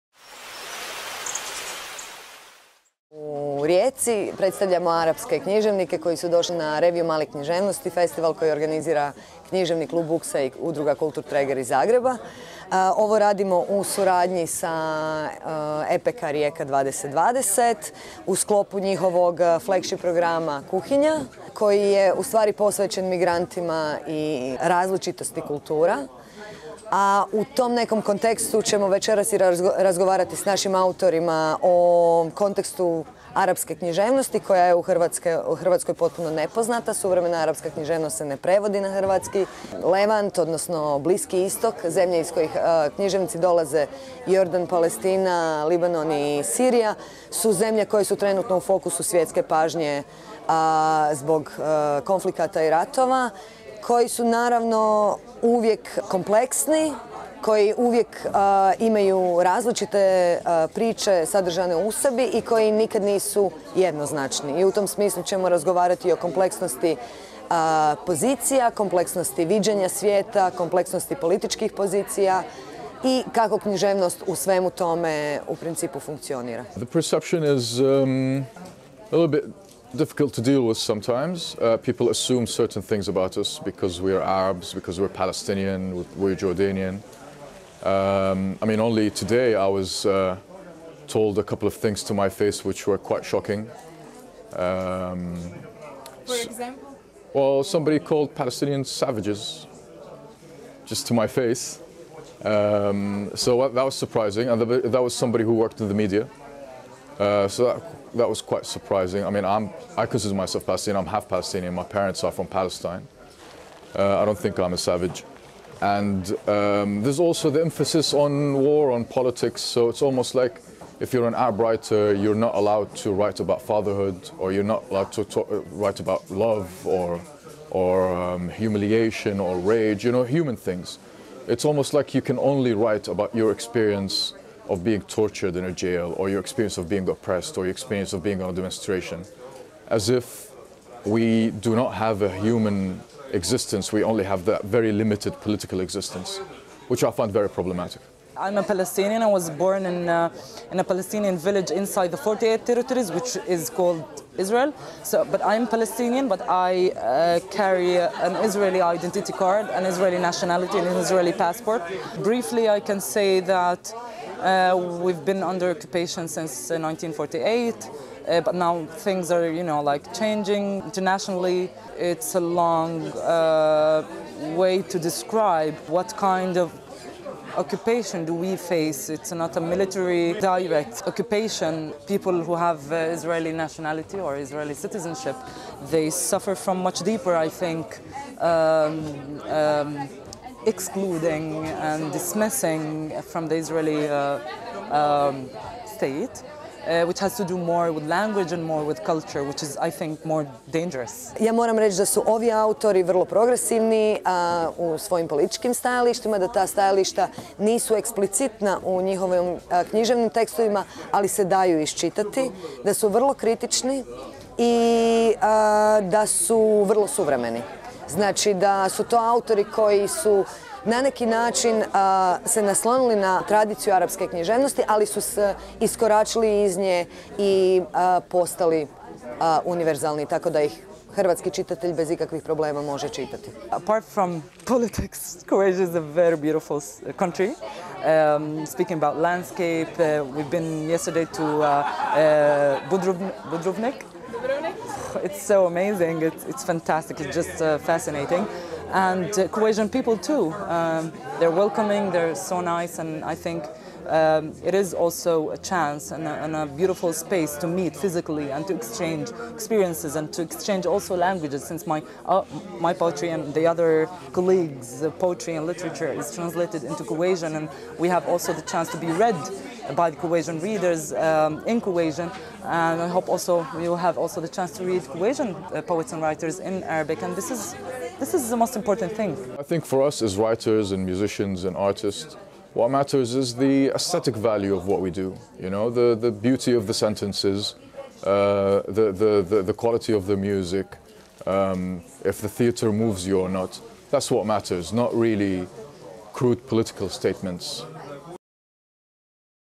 MojaRijeka.hr - Književnici Sirije, Libanona, Jordana i Palestine gostovali u Rijeci
U sklopu Revije malih književnosti, godišnje manifestacije posvećene književnim scenama koje su nepoznate hrvatskoj publici, 8. prosinca, u antikvarijatu Ex Libris, predstavilo se sedmero književnica i književnika iz Sirije, Libanona, Jordana i Palestine.